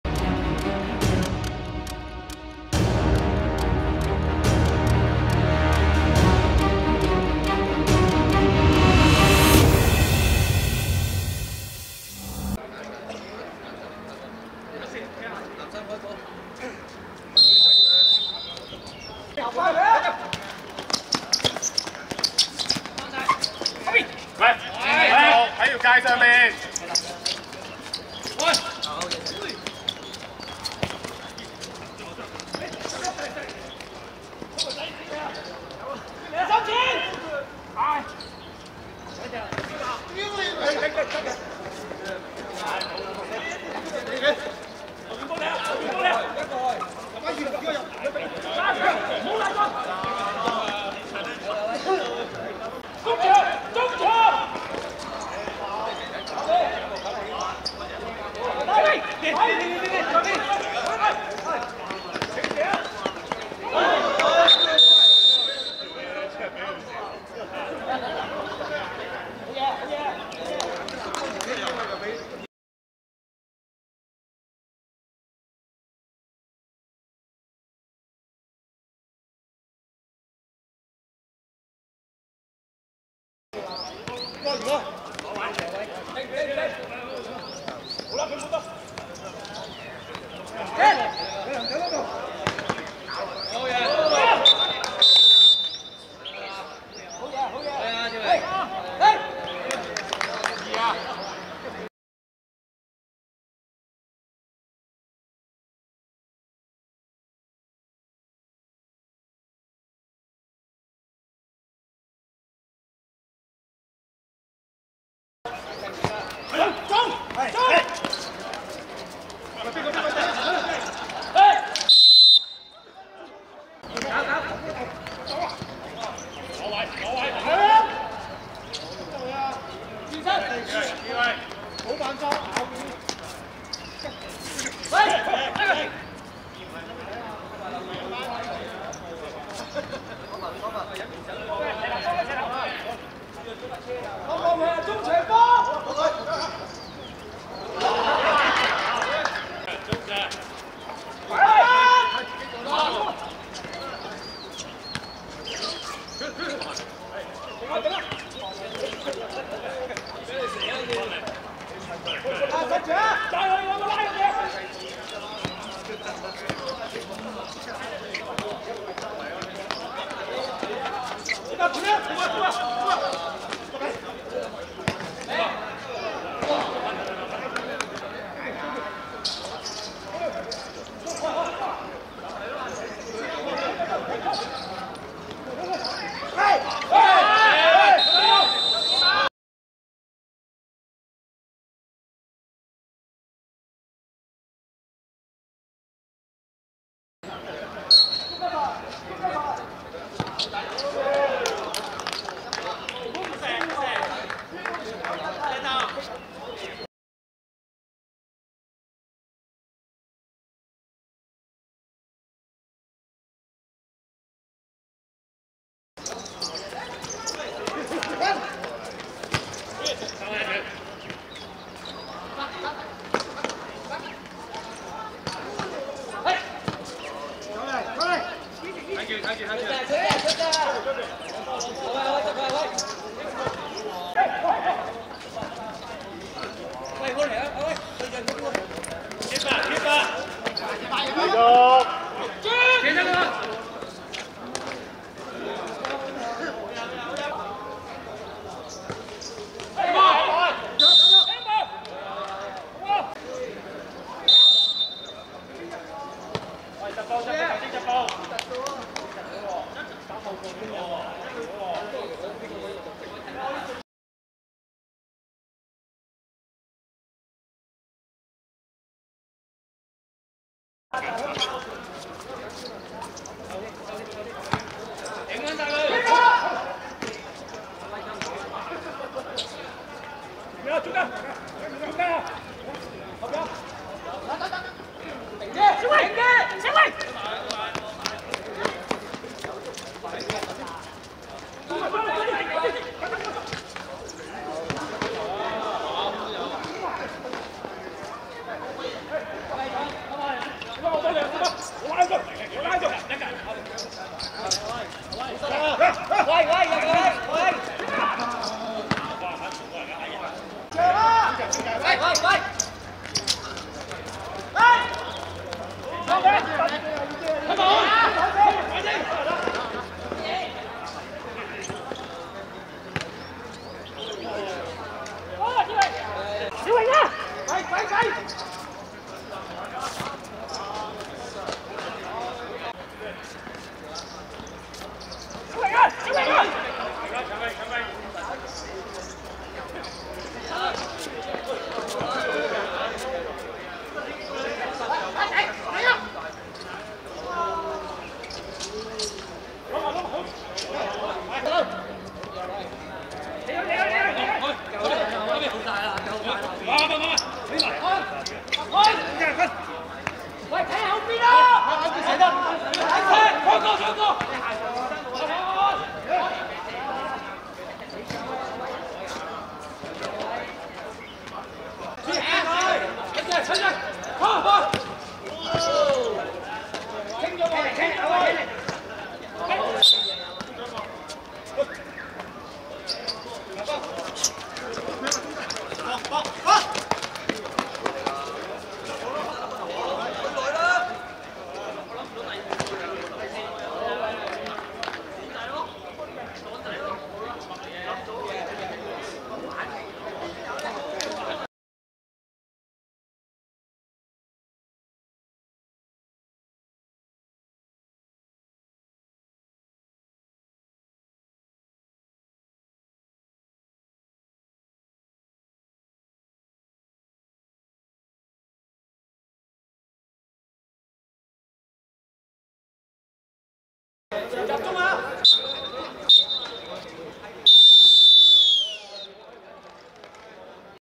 聯盈vs青山聯(2020.3.15.厦村鄉七人足球邀請四角賽~德正堂賀歳盃~次循環)精華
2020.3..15.厦村足球塲 15:20 聯盈(藍) 1 : 4 青山聯(橙)